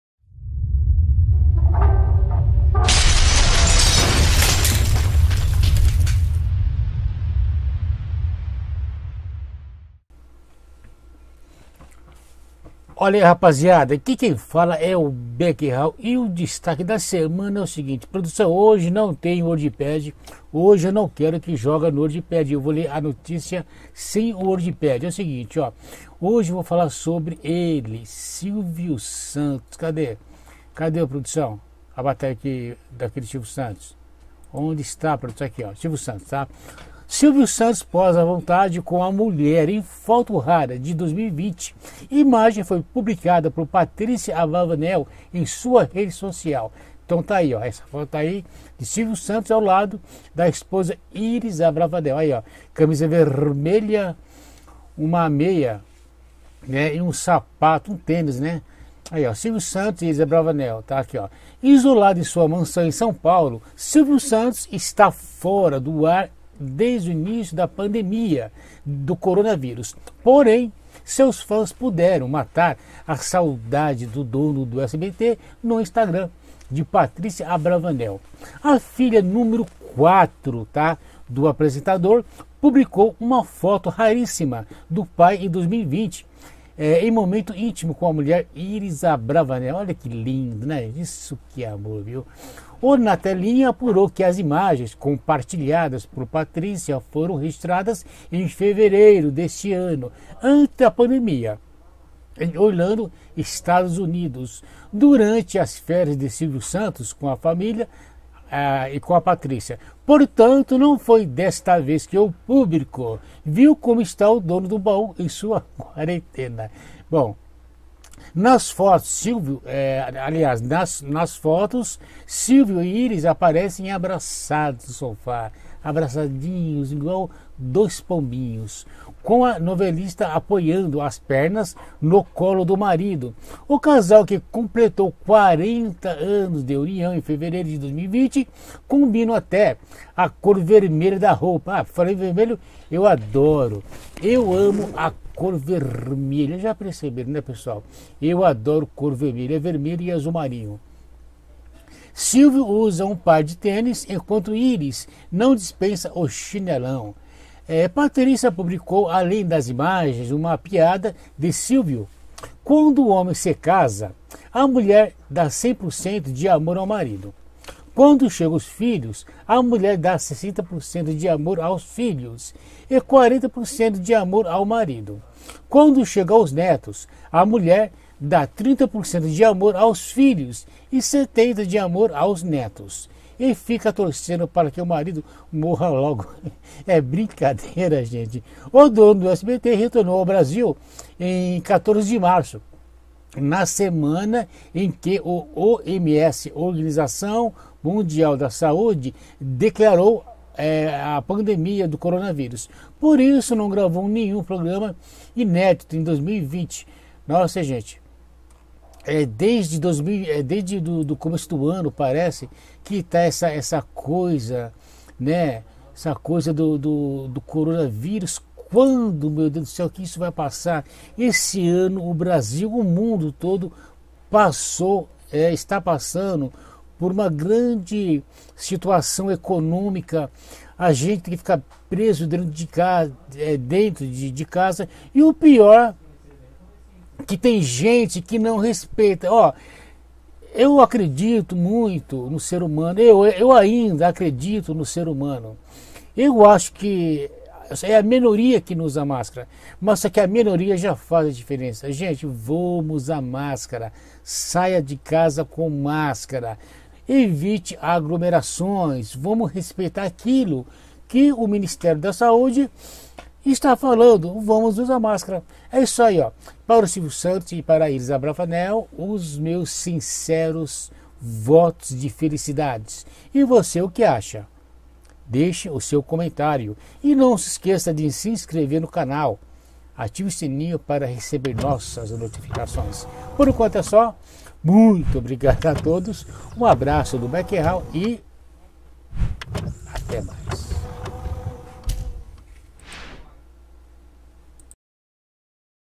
Silvio Santos posa à vontade com a mulher em foto rara de 2020
Devido a pandemia do coronavírus, o apresentador de televisão Silvio Santos não esta realizando novas gravações de seu programa de TV